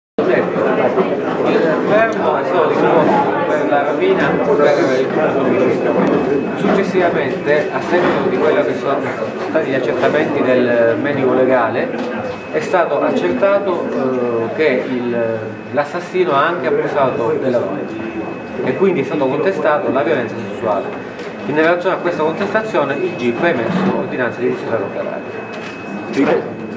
CONIUGI PALAGONIA, IL GIP: L'ASSASSINO HA ABUSATO DELLA DONNA, DISUMANA CRUDELTA'
(Agenzia VISTA) - Caltagirone, 4 Settembre 2015 - Il procuratore di Caltagirone Giuseppe Verzera, incontrando i giornalisti in conferenza stampa, per fare il punto sulle indagini sull'omicidio dei due coniugi Vincenzo Solano e Mercedes Ibanez avvenuto lo scorso 30 agosto a Palagonia, in provincia di Catania. Courtesy TGS